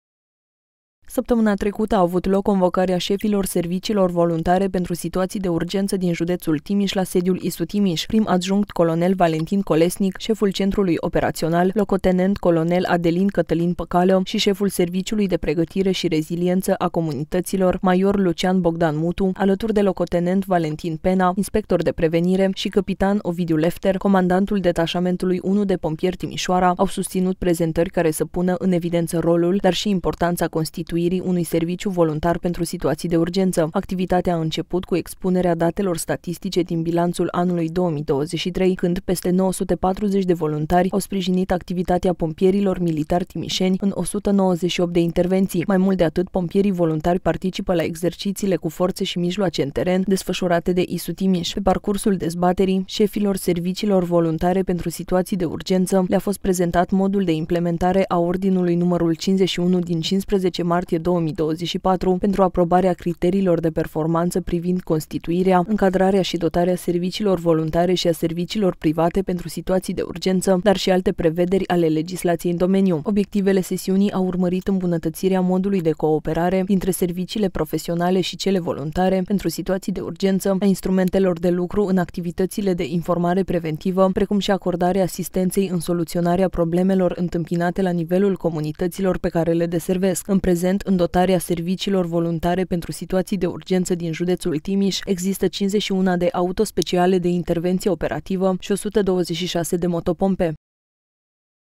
Rolul și importanța constituirii unui serviciu voluntar pentru situații de urgență
A avut loc 𝗖𝗼𝗻𝘃𝗼𝗰𝗮𝗿𝗲𝗮 𝘀̦𝗲𝗳𝗶𝗹𝗼𝗿 𝗦𝗲𝗿𝘃𝗶𝗰𝗶𝗶𝗹𝗼𝗿 𝗩𝗼𝗹𝘂𝗻𝘁𝗮𝗿𝗲 𝗽𝗲𝗻𝘁𝗿𝘂 𝗦𝗶𝘁𝘂𝗮𝘁̦𝗶𝗶 𝗱𝗲 𝗨𝗿𝗴𝗲𝗻𝘁̦𝗮̆ 𝗱𝗶𝗻 𝗷𝘂𝗱𝗲𝘁̦𝘂𝗹 𝗧𝗶𝗺𝗶𝘀̦ la sediul inspectoratului. Prim-adjunct, colonel Valentin COLESNIC, șeful Centrului Operațional, lt.col. Adelin-Cătălin PĂCALĂ și șeful Serviciului de Pregătire și Reziliență a Comunităților, mr. Lucian-Bogdan MUTU, alături de lt. Valentin PENA, Inspector de prevenire și cpt. Ovidiu LEFTER, comandantul Detașamentului 1 de Pompieri Timișoara, au susțin prezentări care să pună in evidență rolul, dar și importanța constituirii unui serviciu voluntar pentru situații de urgență. Activitatea a început cu expunerea datelor statistice din bilanțul anului 2023 când, peste 940 de voluntari au sprijin activitatea pompierilor militari timișeni în 198 de intervenții. Mai mult de atât, pompierii voluntari participă la exercițiile cu forțe și mijloace în teren desfășurate de ISU Timiș.